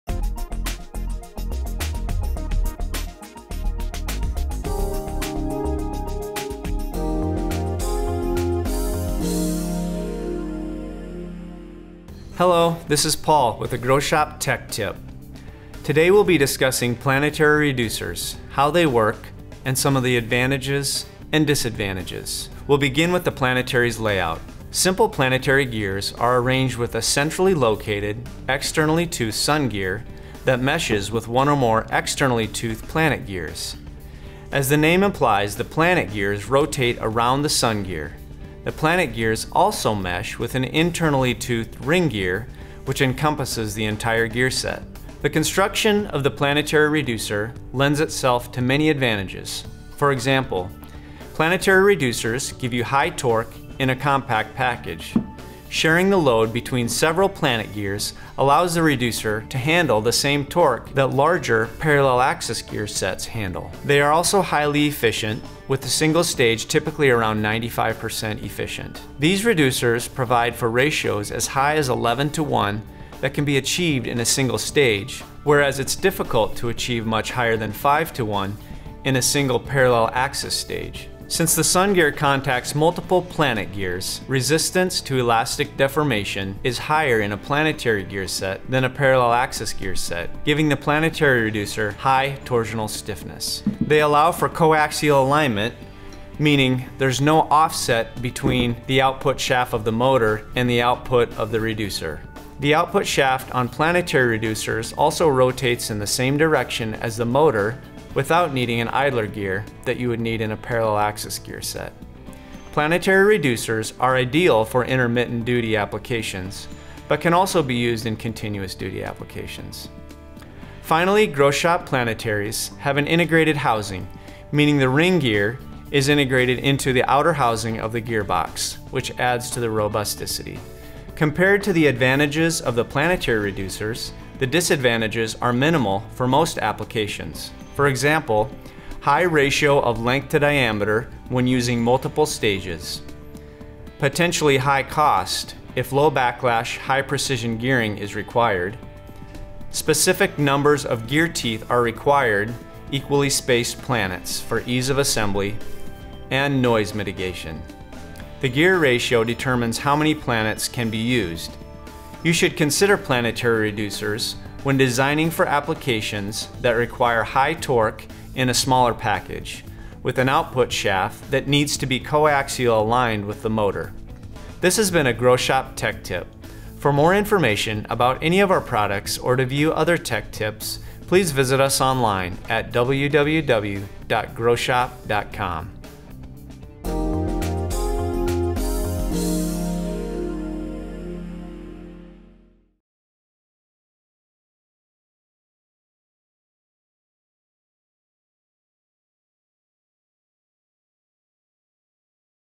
Groschopp Tech Tips | Planetary Gearboxes
In this video we discuss planetary gearboxes. Learn the ins and outs of how these reducers work as well as their advantages and disadvantages.